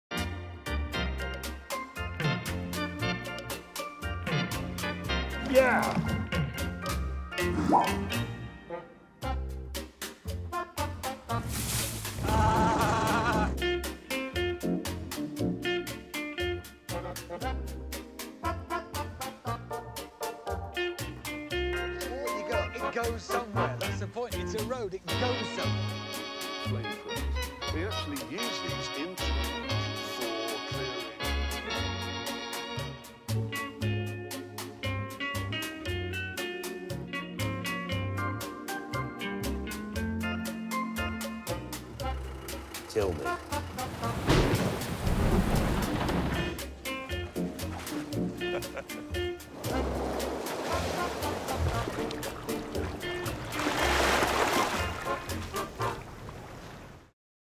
Just James May Vibing For Nearly A Minute 😎
We should all aspire to be as absolutely chill as James May is in every single situation.